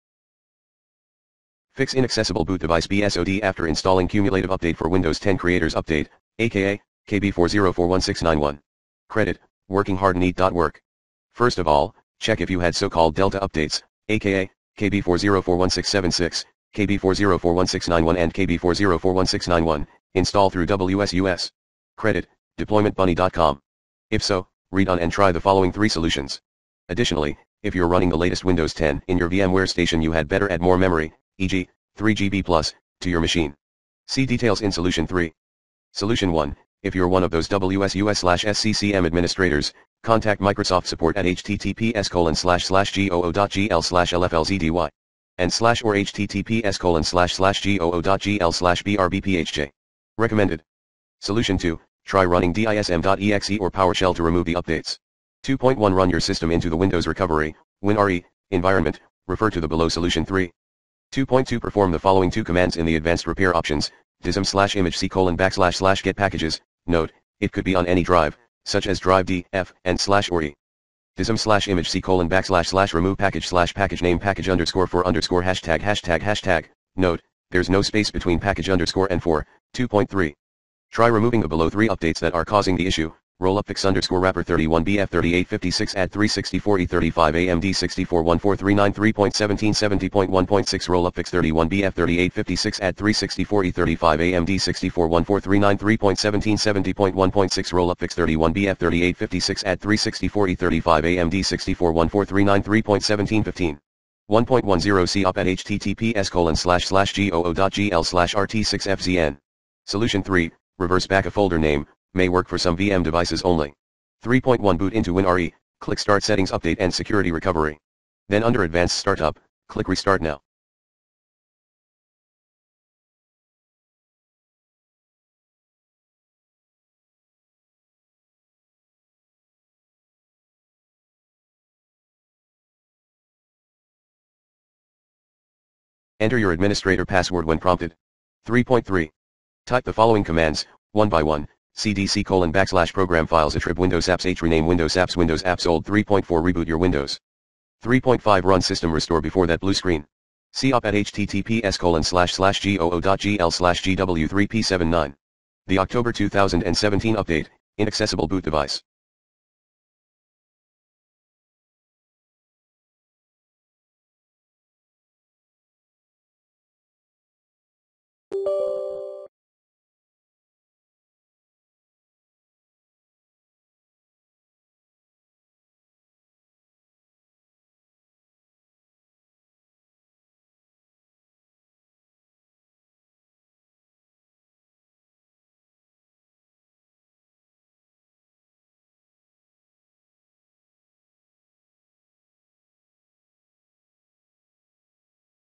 Fix Inaccessible Boot Device BSOD after installing Cumulative Update for Windows 10 Creators Update
This vid helps Fix Inaccessible Boot Device after you installed Cumulative Update for Windows 10 Creators Update (KB4041691). Learn more?

Fix Activation error while updating Norton Internet Security